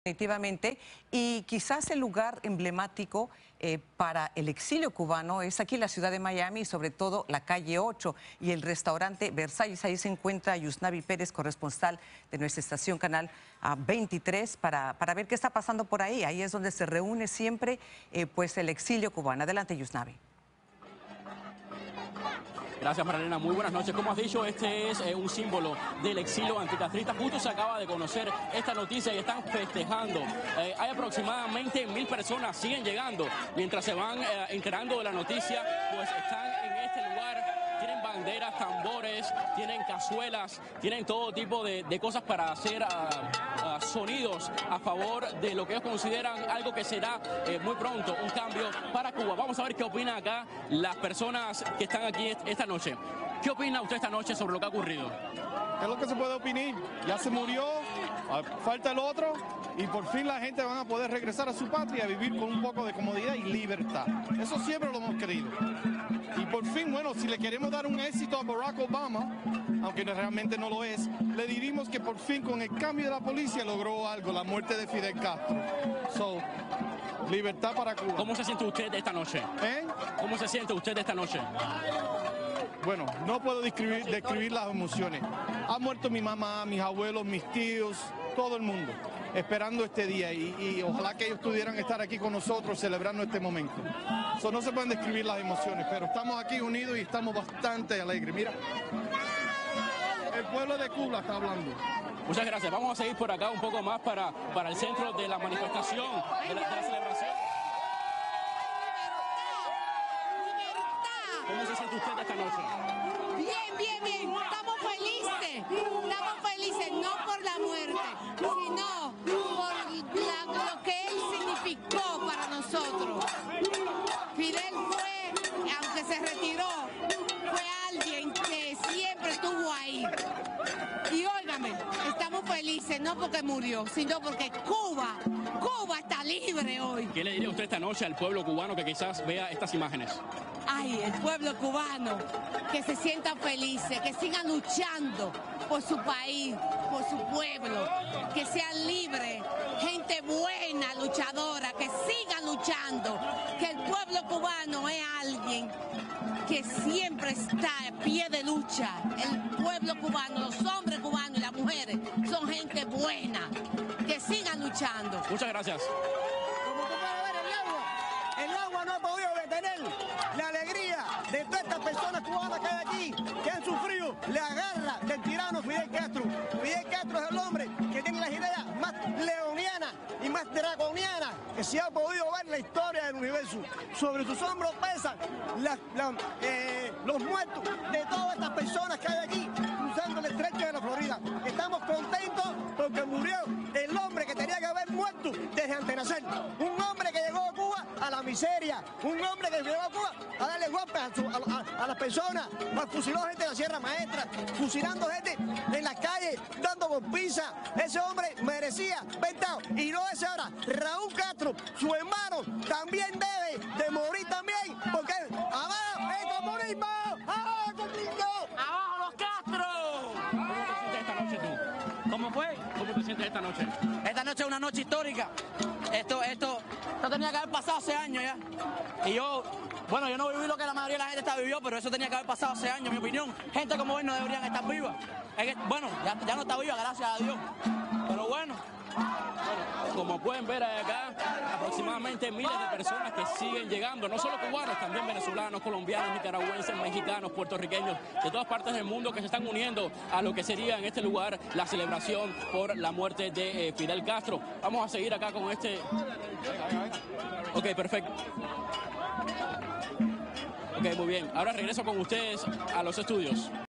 Al menos 1,000 personas celebran en la Pequeña Habana la muerte de Fidel Castro
Ciudadanos festejan con tambores y banderas en la calle 8 de Miami que por fin decenas de cubanos van a poder regresar a su patria y vivir en libertad.